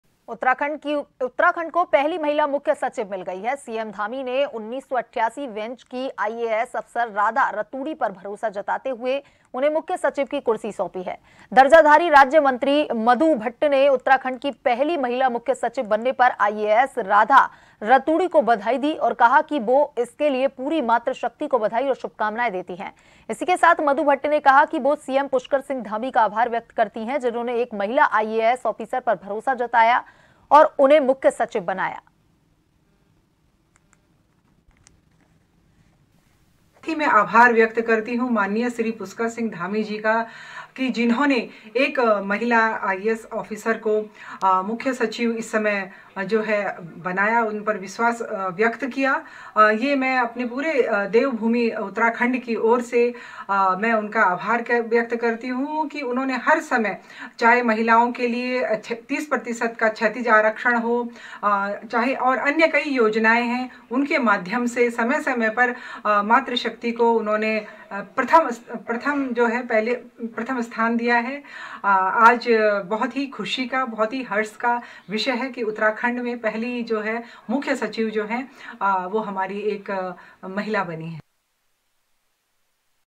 Dehradun News | पहली महिला मुख्य सचिव बनने का मिला गौरव, IAS राधा रतूड़ी के कंधो पर उत्तराखंड की कमान
Watch JAN TV on :
Tata Play DTH : 1185
Airtel DTH: 355
JIO Fiber: 1384